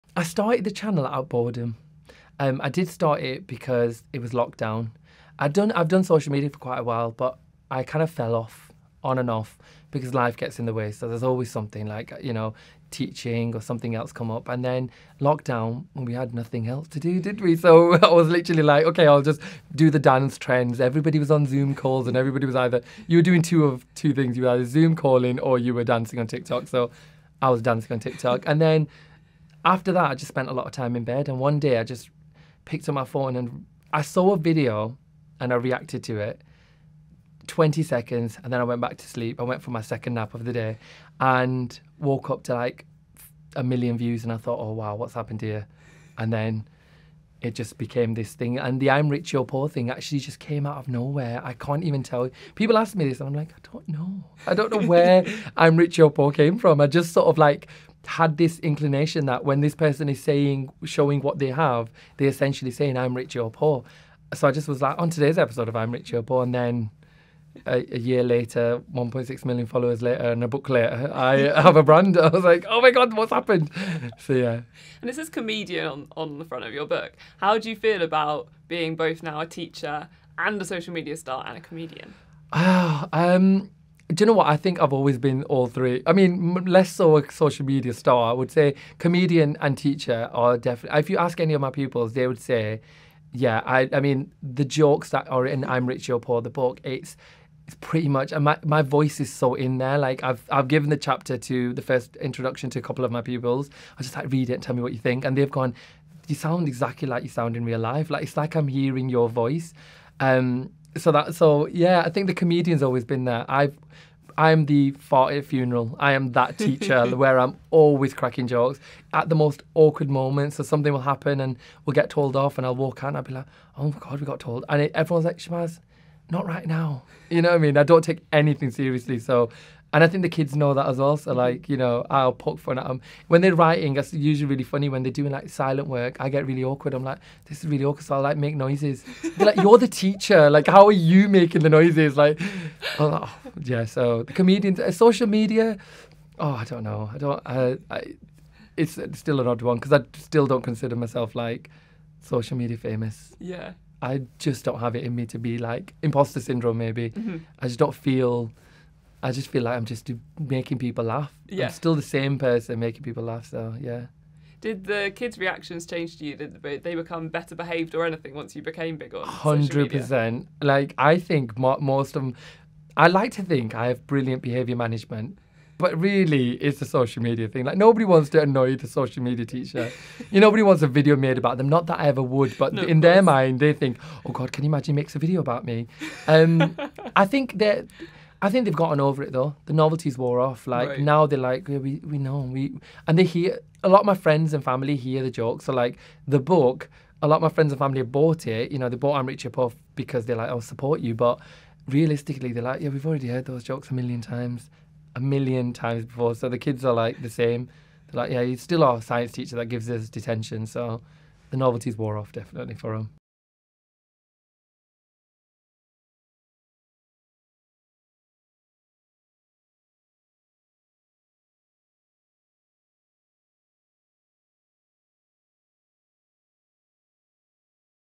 How Shabaz Ali Became Tik Tok Famous | Audible Sessions
Shabaz Ali talks about how he started his channel during lockdown when people were either on Zoom or dancing on Tiktok (he was the latter).